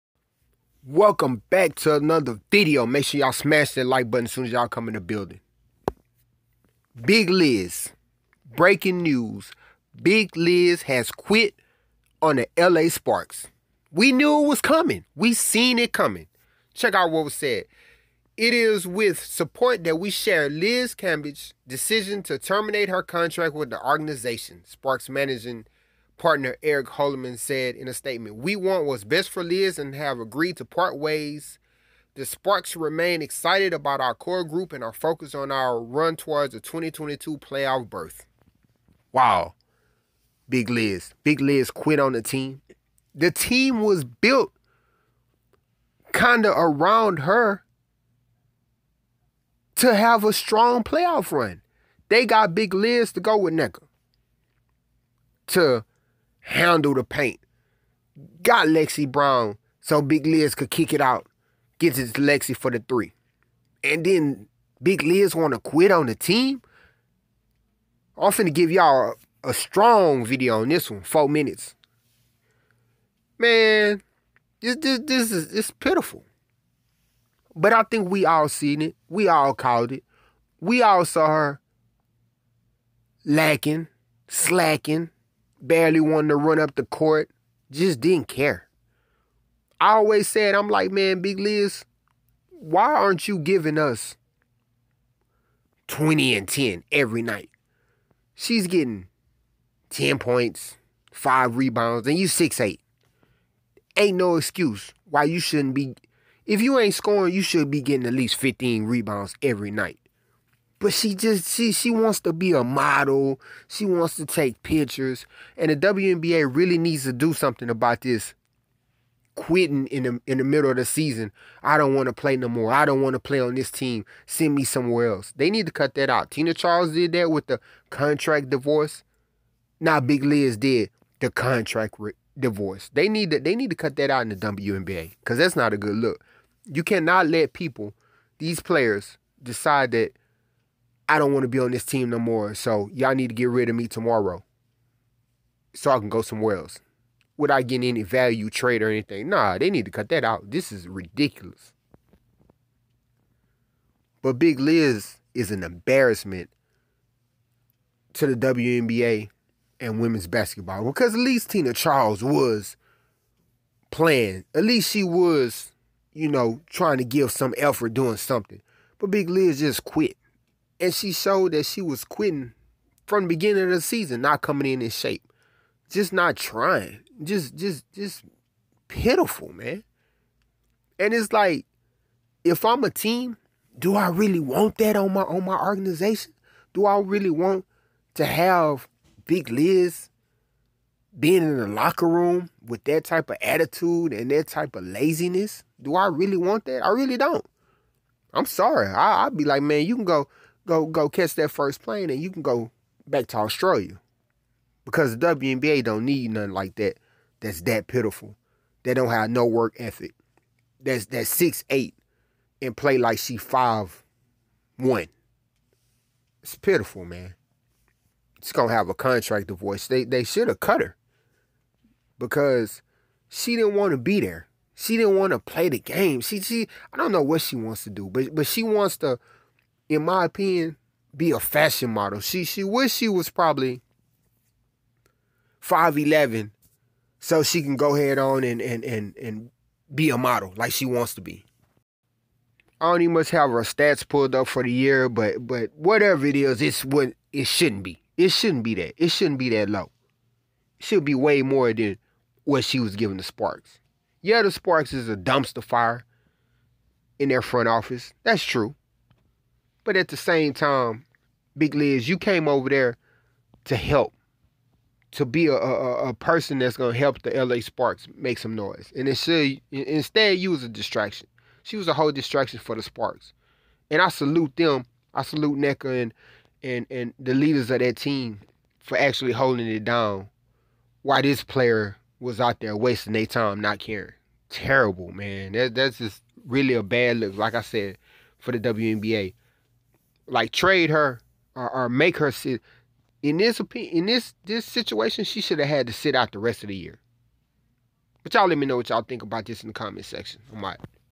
Breaking News‼️ Liz Cambage Quits On The LA Sparks 👀
Big Liz wants out of la and has ended her contract with the sparks

She did a Tina Charles and announced a contract divorce from the sparks

The WNBA needs to do something about this